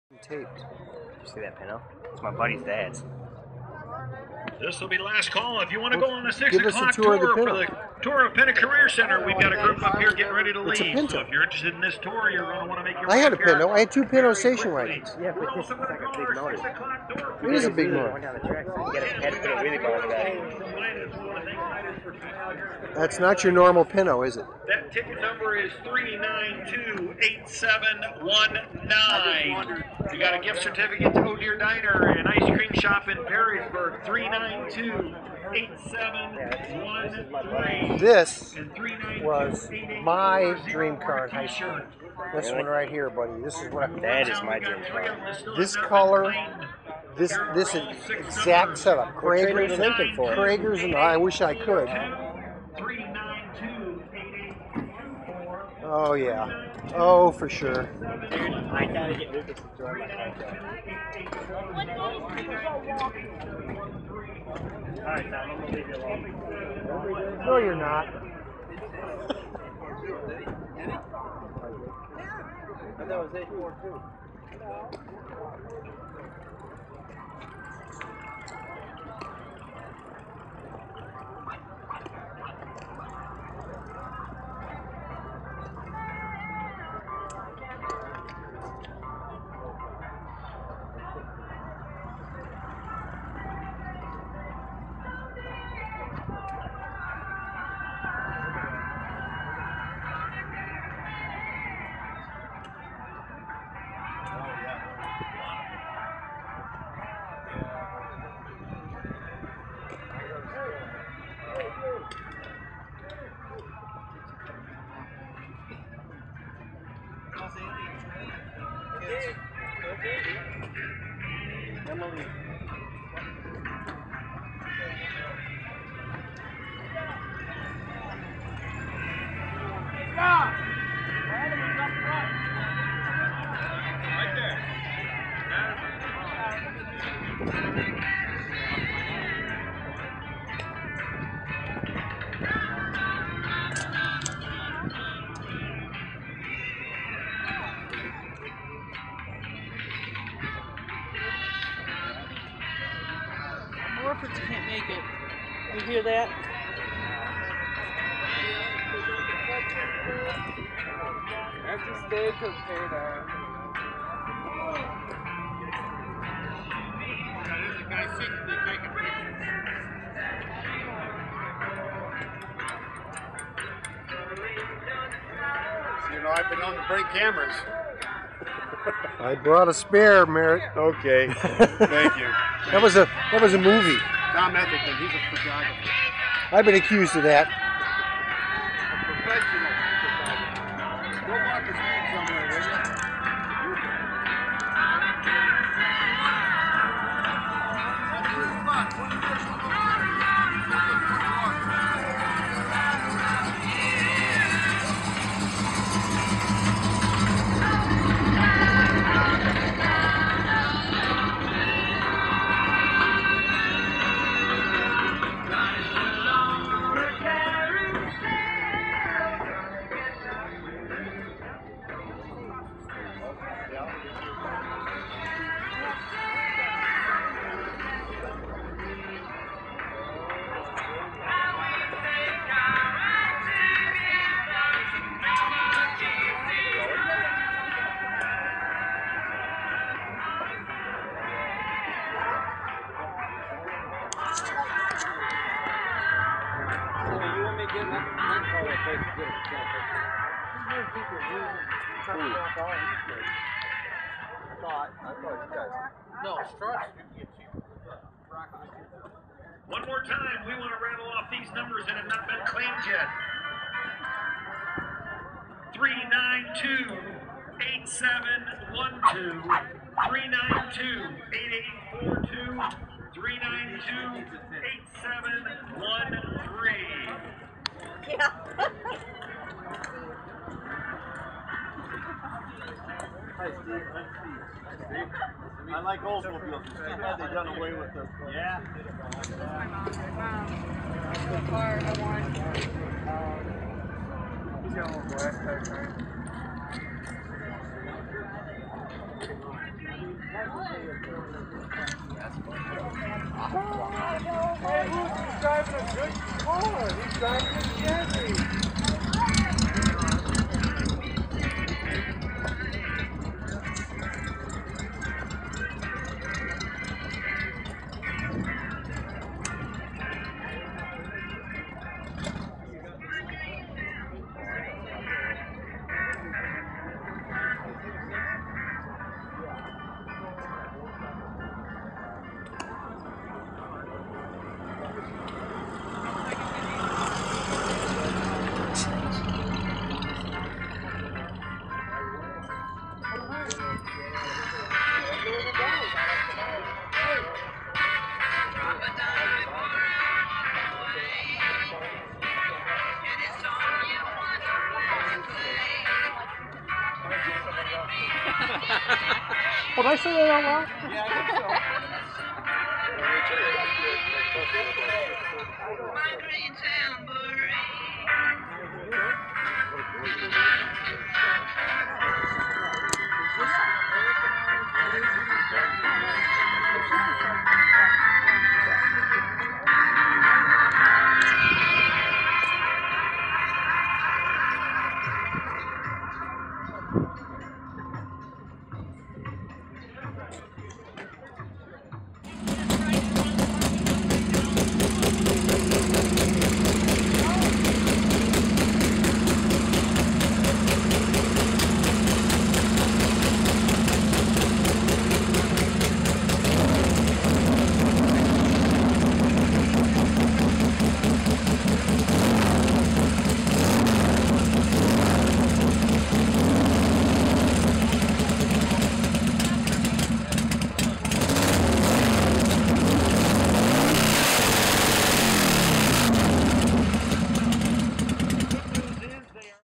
PENTA Car Show 9 25 13
The PENTA Automotive Program Annual Car Show September 25th 2013